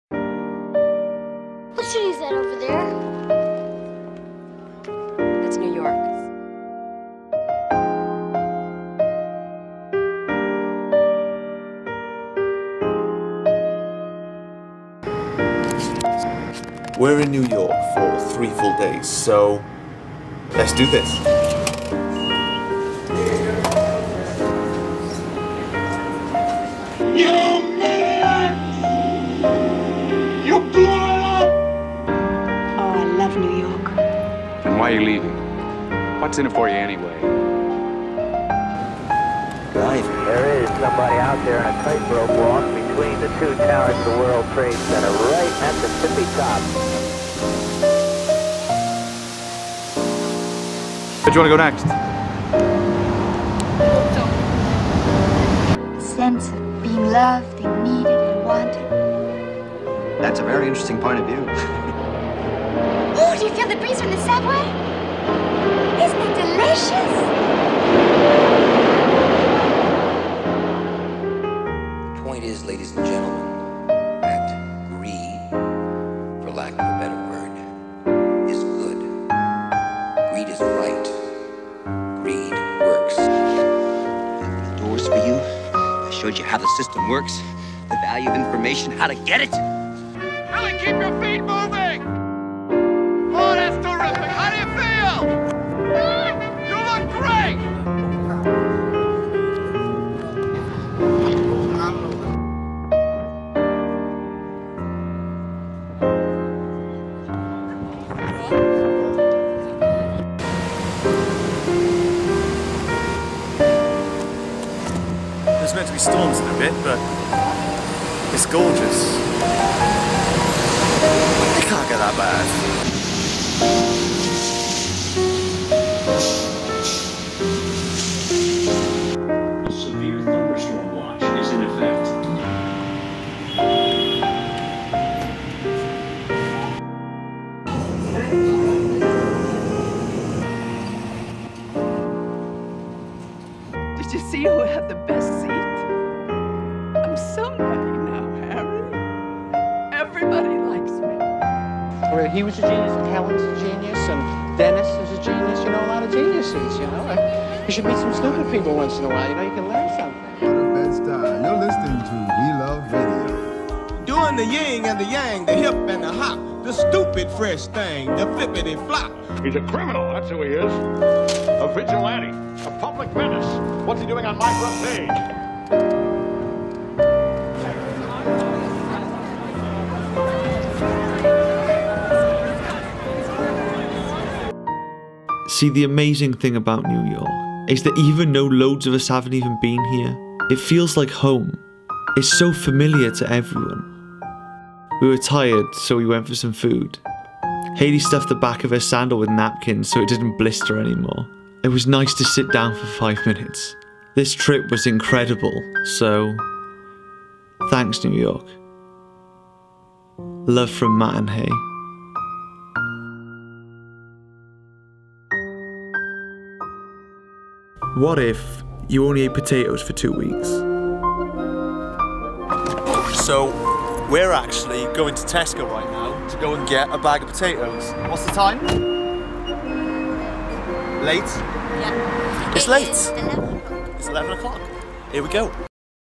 New York Film Locations
I've never been to a place like New York before, the nostalgia was so strong for somewhere I hadn't been before